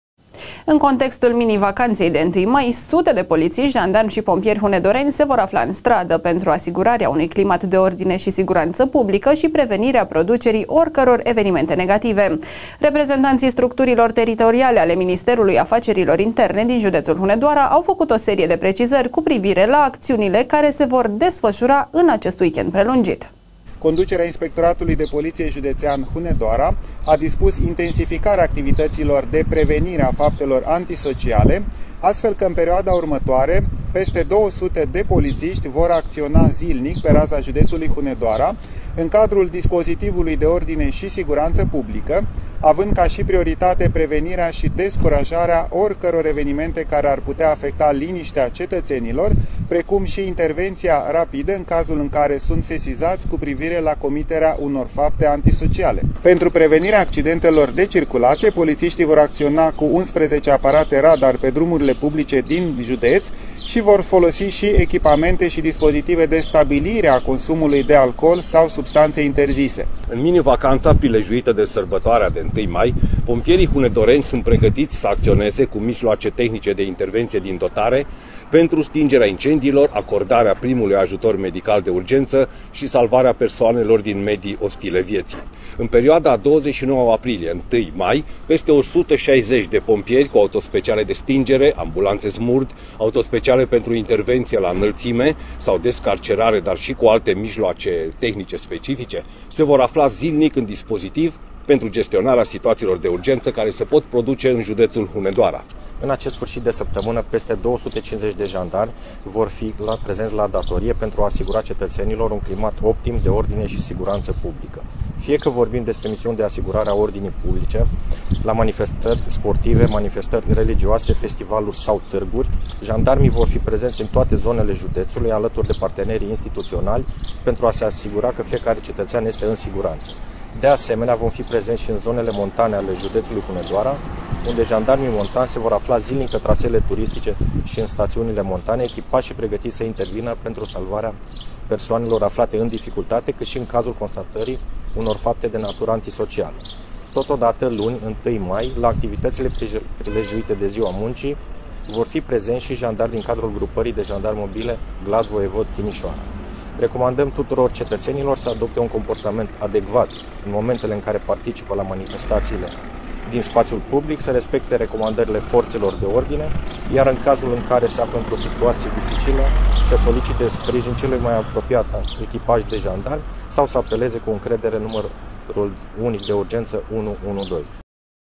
DE 1 MAI, MINIVACANȚĂ ÎN SIGURANȚĂ!
SUTE DE POLIȚIȘTI, JANDARMI ȘI POMPIERI MOBILIZAȚI PENTRU ORDINE ȘI SIGURANȚĂ PUBLICĂ
      În contextul minivacanței de 1 Mai, sute de poliţişti, jandarmi și pompieri hunedoreni se vor afla în stradă, pentru asigurarea unui climat de ordine şi siguranţă publică şi prevenirea producerii oricăror evenimente negative. Reprezentanții  structurilor teritoriale ale Ministerului Afacerilor Interne din județul Hunedoara au făcut o serie de precizări cu privire la acțiunile care se vor desfășura în acest weekend prelungit